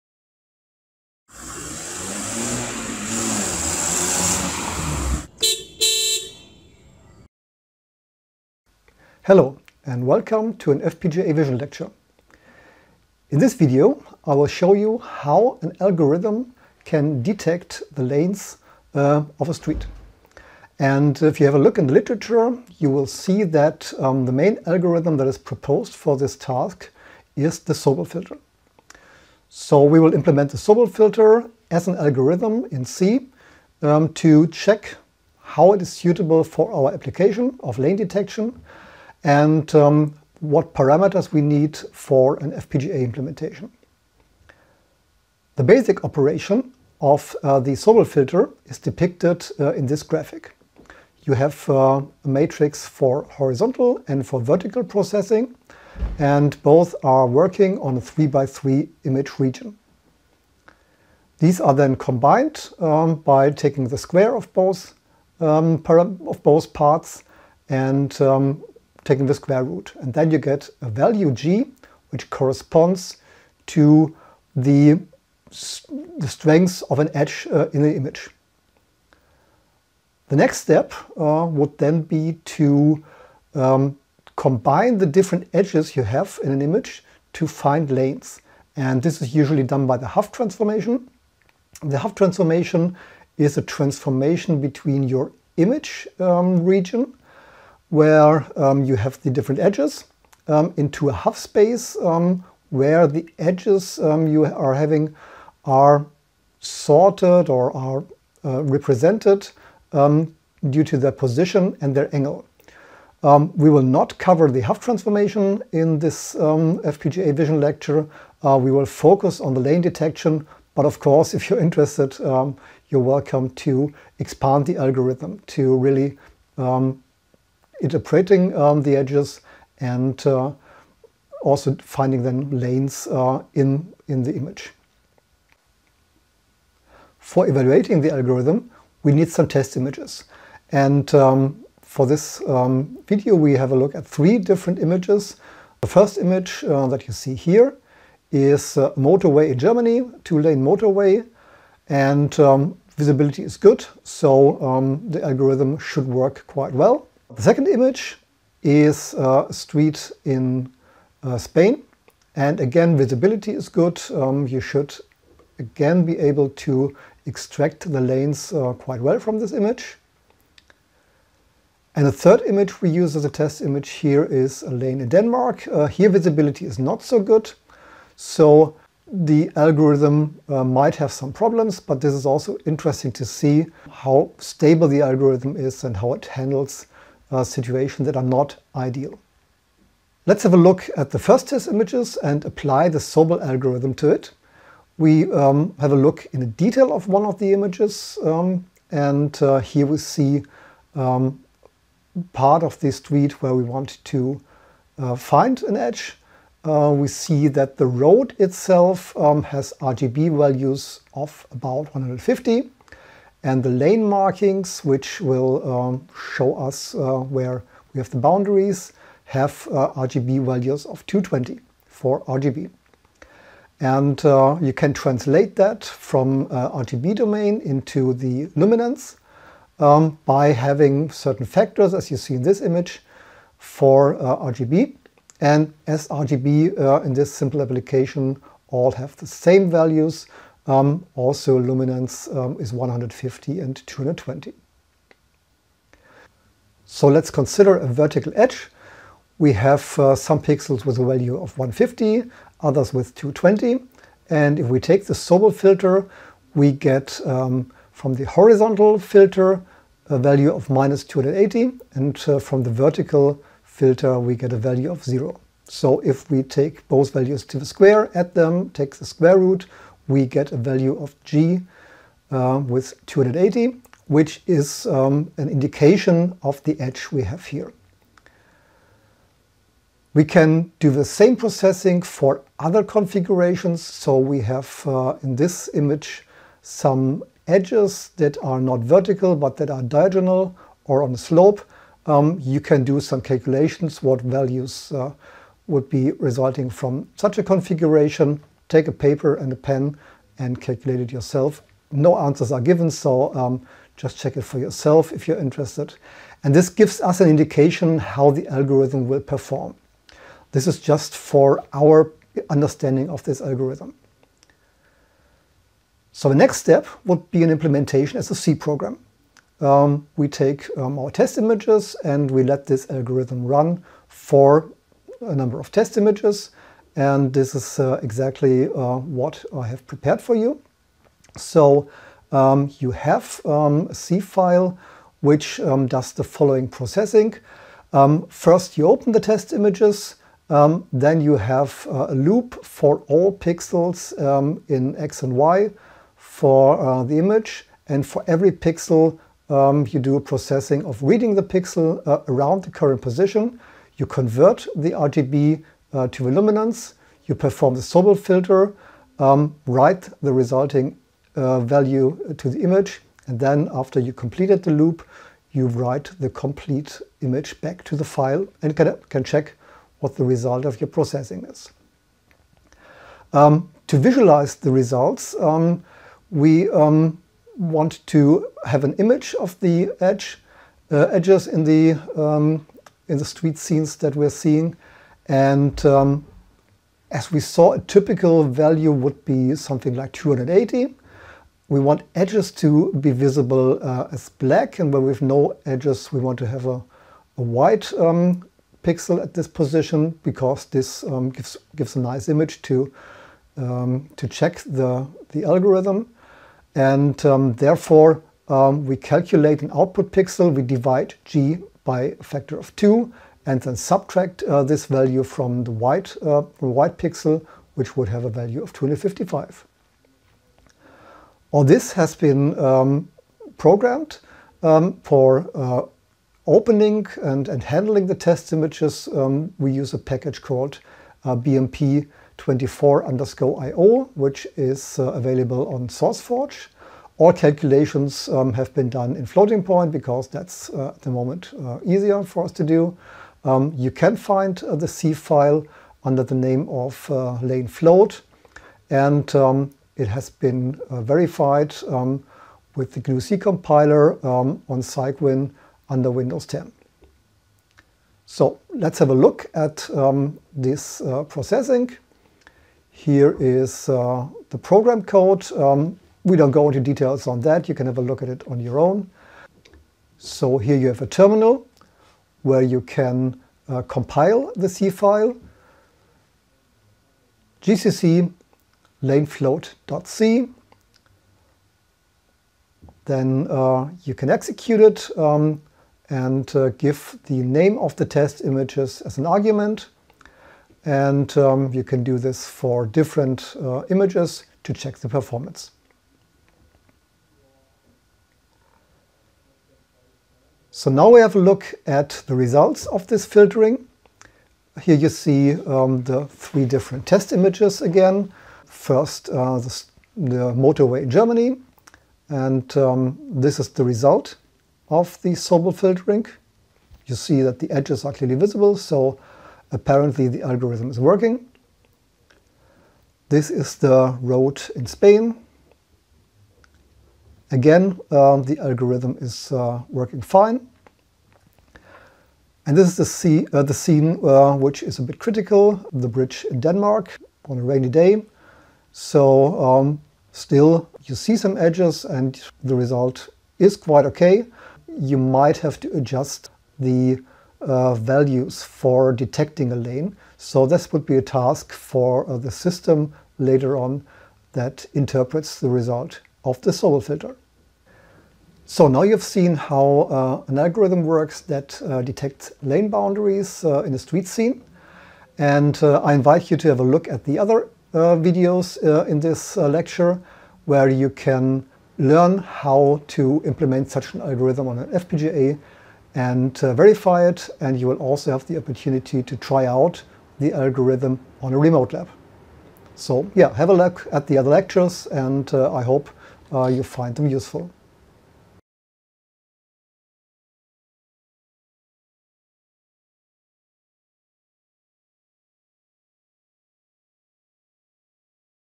FPGA Vision - Lane Detection
Remote Lecture on an FPGA-Implementation of Lane Detection
- Algorithm and verification
- Implementation as C program

Marco Winzker, Professor for Digital Design